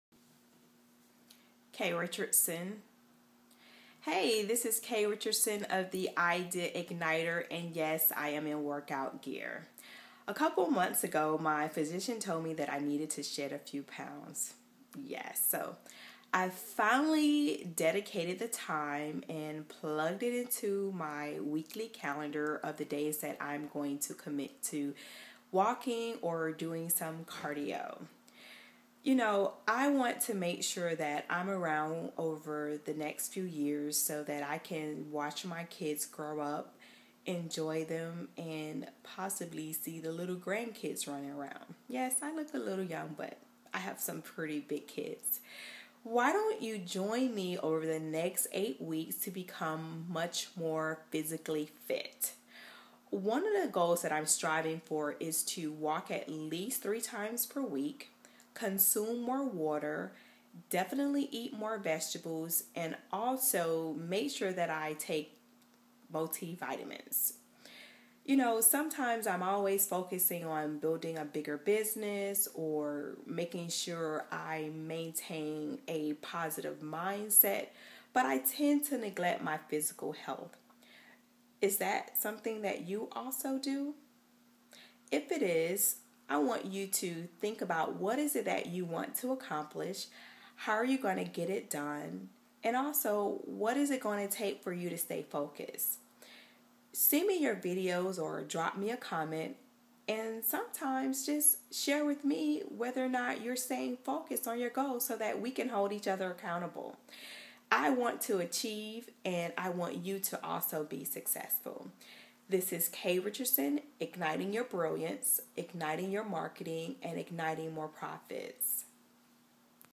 8 weeks get fit challenge
I'm on a journey to becoming physically fit. Have you pushed exercising to the side to work on your passion?  Join me and lets build businesses  as healthier bodies.